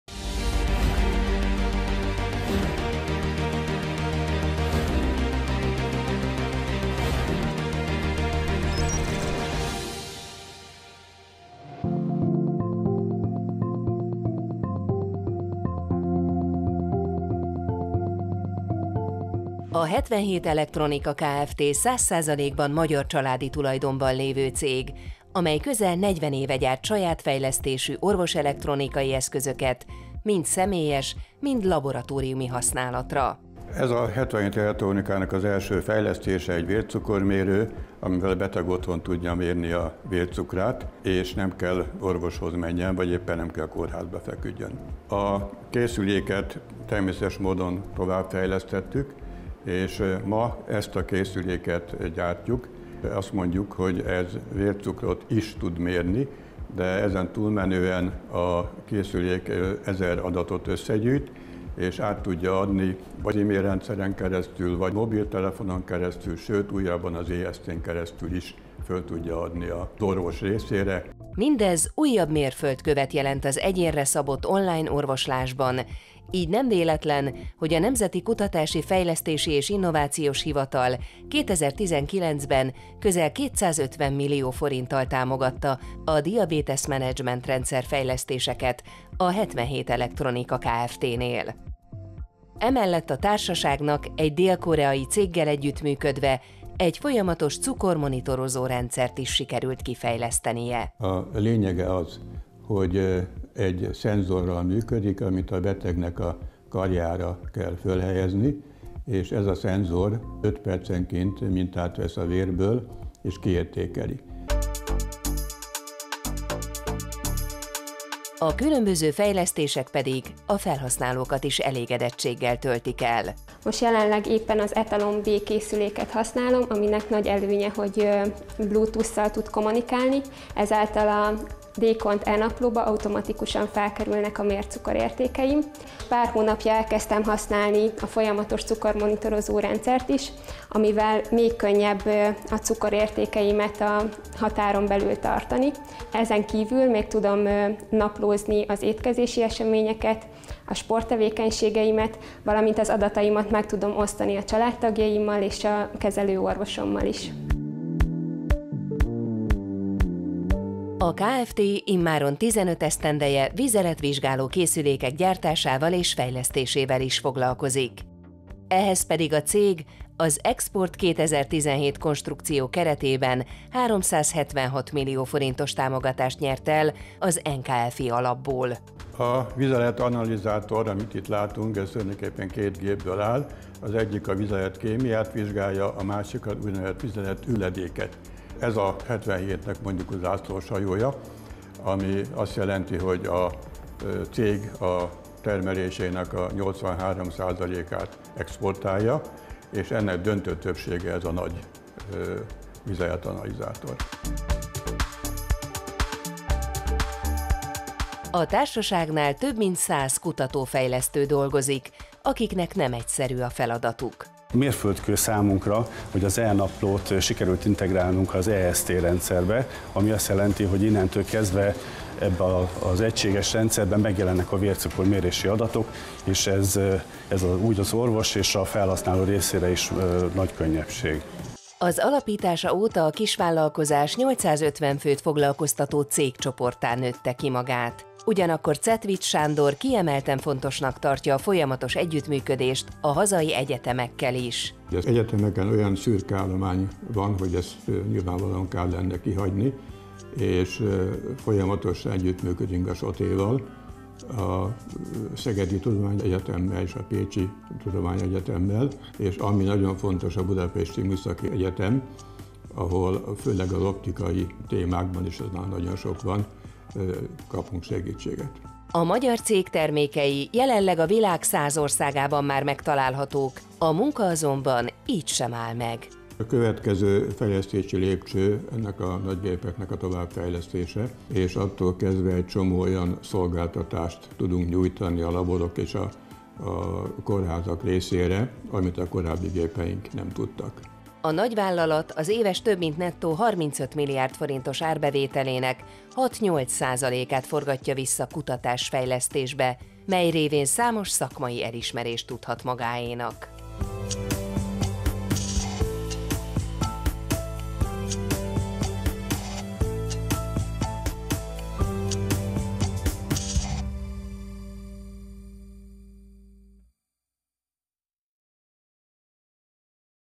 Sikertörténetek: 77 Elektronika Kft.
Ismét egy magyar fejlesztésű telemedicinás rendszer forradalmasíthatja az online orvosi távszolgáltatást. A 77 Elektronika Kft. projektjének célja a diabéteszes, vagy annak előszobájában lévő páciensek személyre szabott kezelése. (2024)